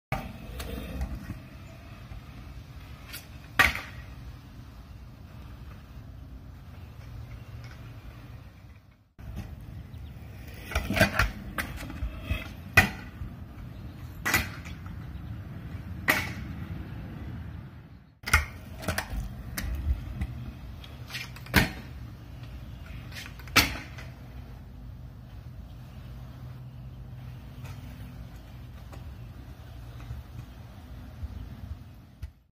Clips 7/3 on new scooter
Trying to get back into it. Nothing special here, just getting the muscle memory back.